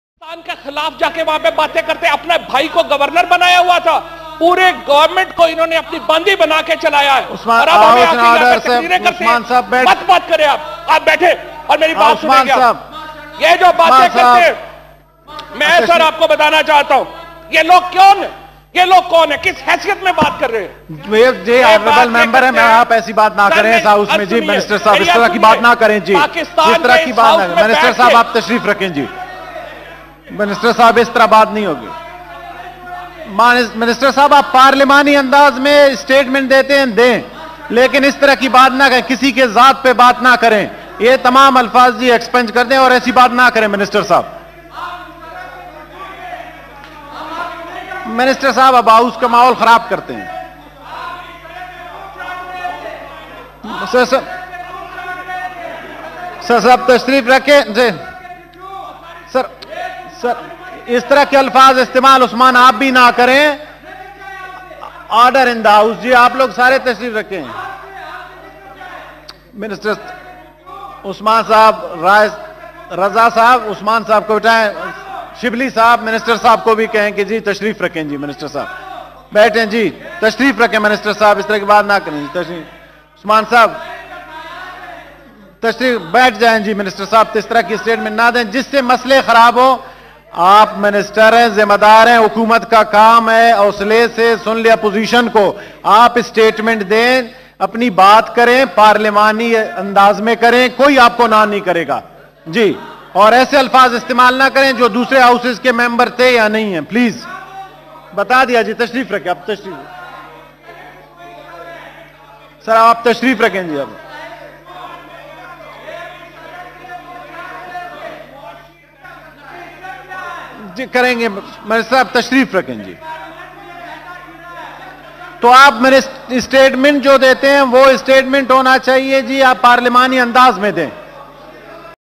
Fawad Chaudhry Taking Class Of Opposition In Senate Today | 14th November 2018
Fawad Chaudhry Speech in Senate today | 14th November 2018